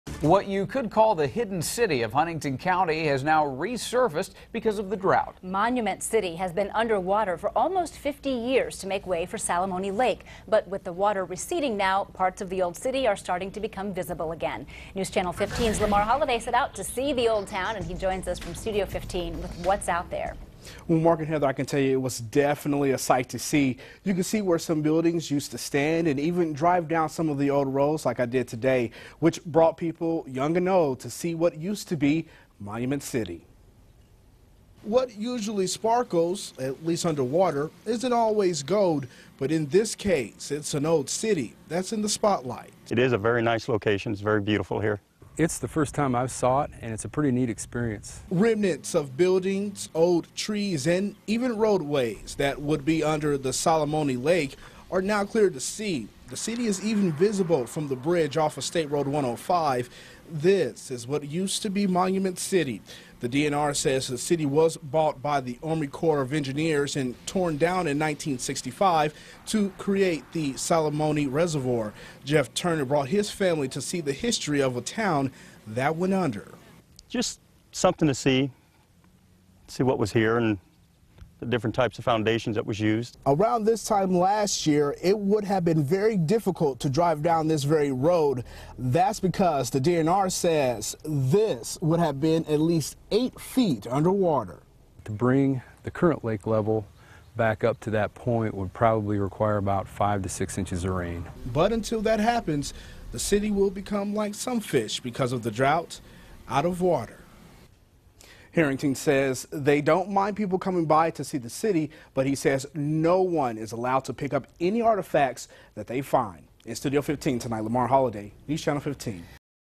Monument City resurfaces from Salamonie Lake
Due to the drought, water at the Salamonie Lake is receding, causing Monument City to resurface.